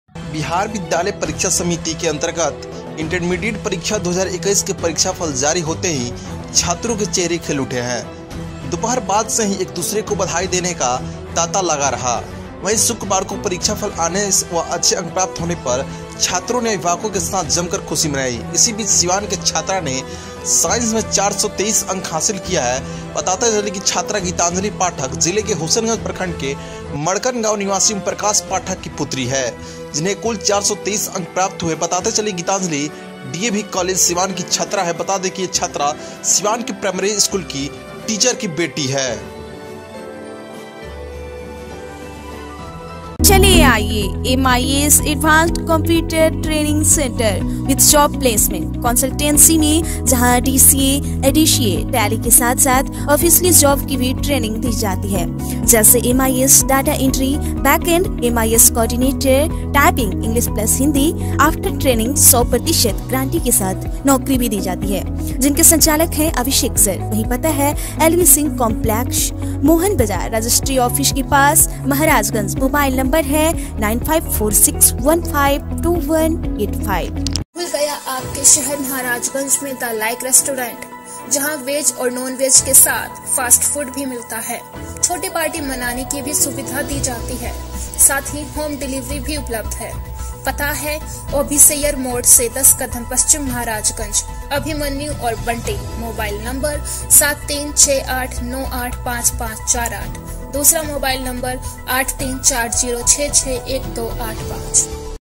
इस लड़की ने बिहार बोर्ड इंटर परीक्षा में सिवान जिले का किया नाम रोशन - A1NEWSBIHAR.IN
Follow A1 Newsbihar Twitter :- https:/p/
Like A1 Newsbihar Facebook :- ?id=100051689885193

Produced By :- A1NEWSBIHAR

Edited By :- Mr. Prakash Kumar